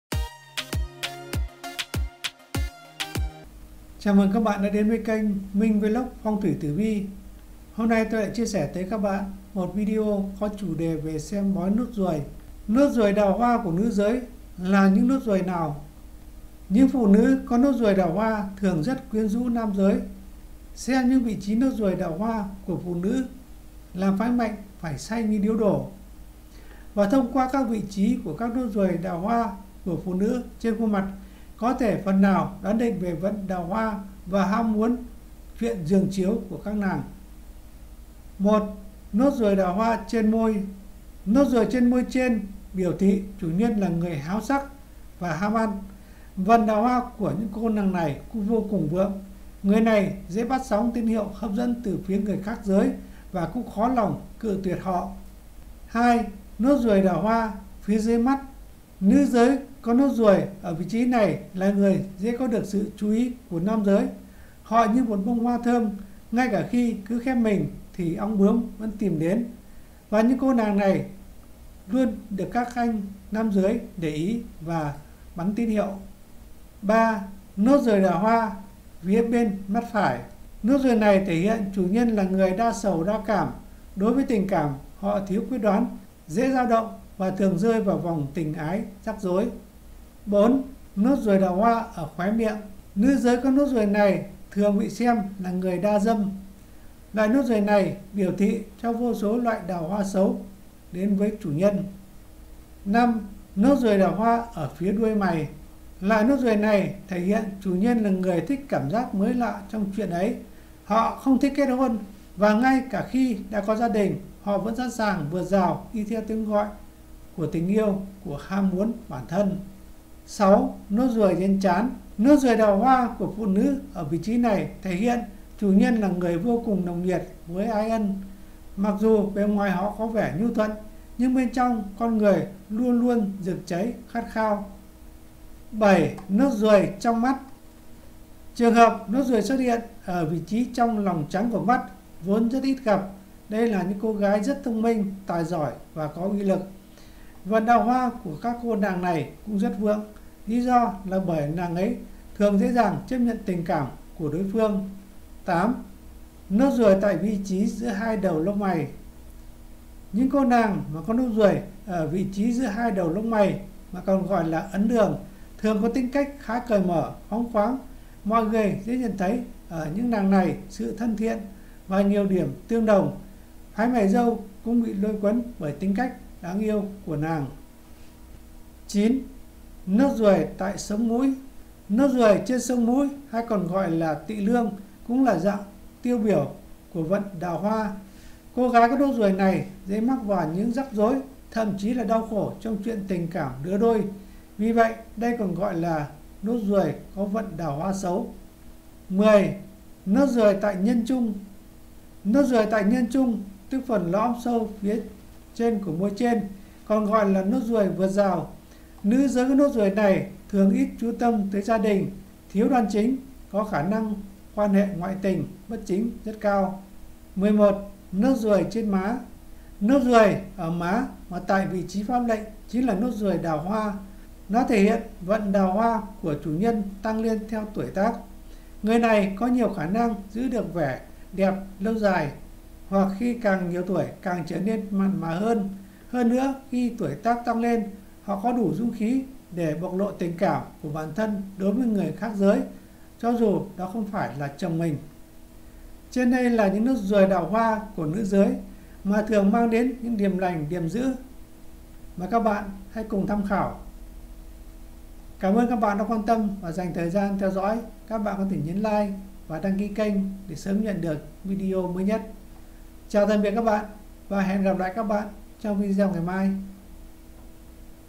Nốt ruồi đào hoa của nữ giới là những nốt ruồi nào | Xem bói nốt ruồi | Nhân tướng học
Nốt ruồi mọc ở những vị trí dưới đây dự đoán cho đào hoa vượng của các cô nàng. Hãy xem đó là những vị trí nào?